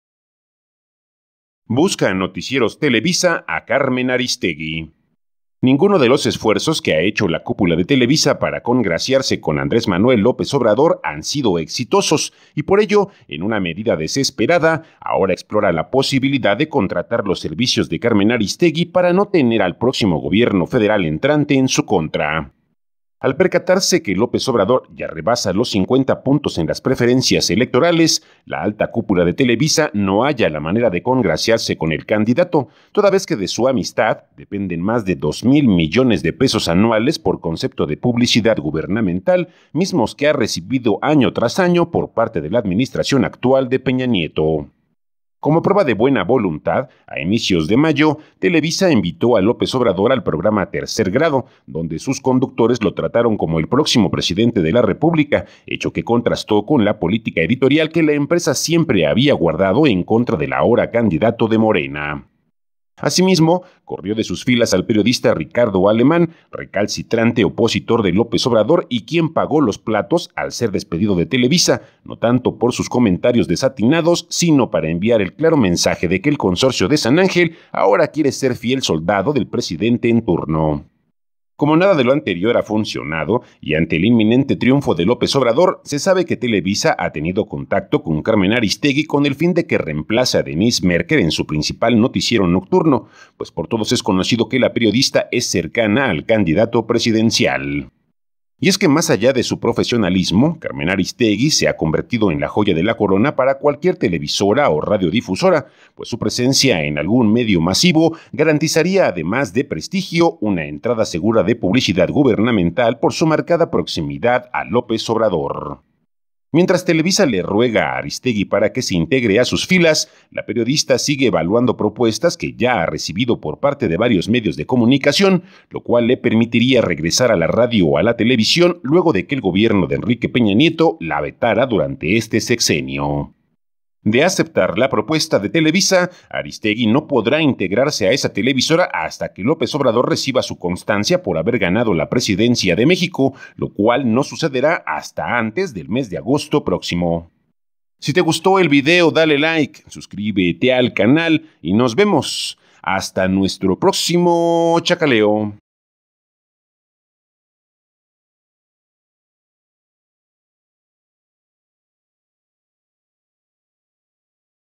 Busca Noticieros Televisa a Carmen Aristegui
Ninguno de los esfuerzos que ha hecho la cúpula de Televisa para congraciase con Andrés Manuel López Obrador han sido exitosos, y por ello, en una medida desesperada, ahora explora la posibilidad de contratar los servicios de Carmen Aristegui para no tener al próximo gobierno federal entrante en su contra.